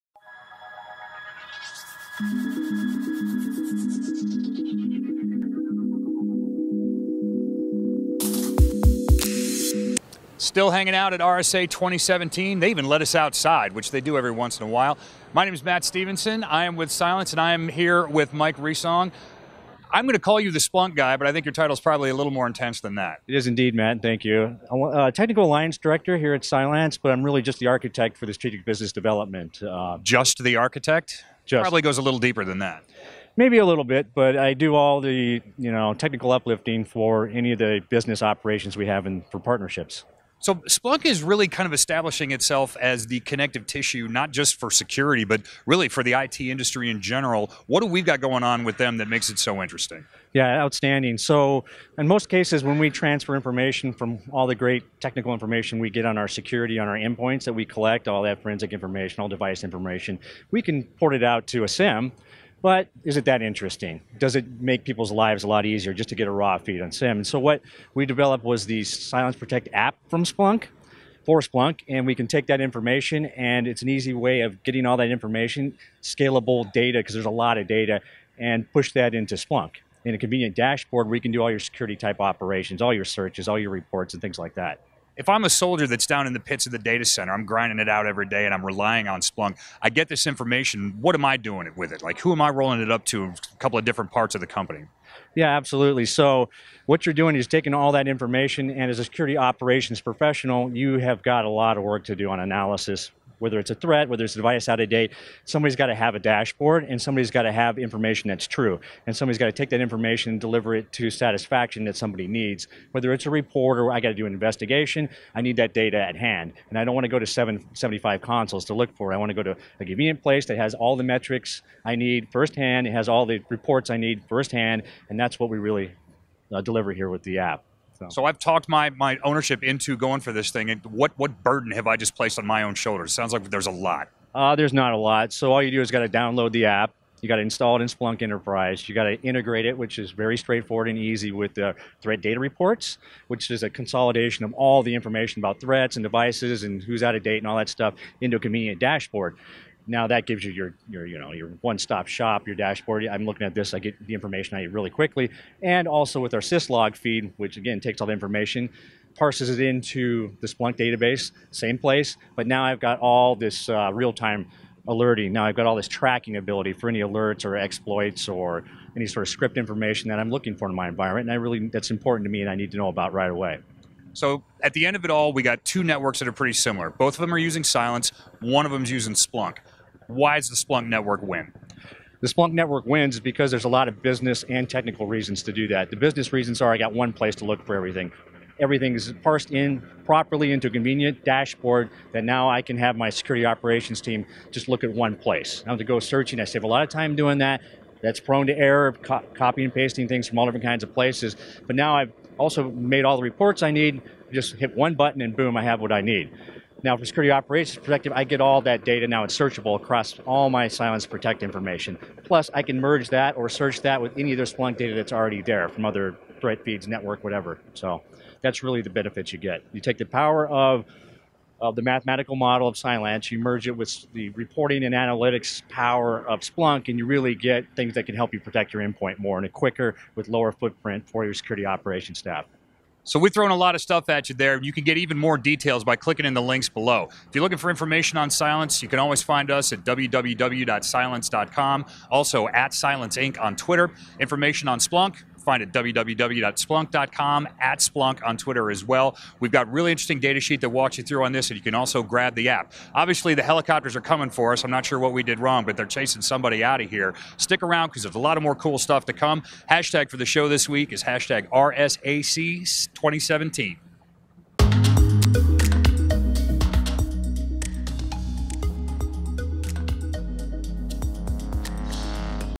Cylance's Splunk Architect Mike Resong Breaks Down the Cylance/Splunk Integration
Cylance's Technical Alliance Director Mike Resong digs into the guts of the Cylance/Splunk integration.

Stay up to date by following @cylanceinc and RSAC2017.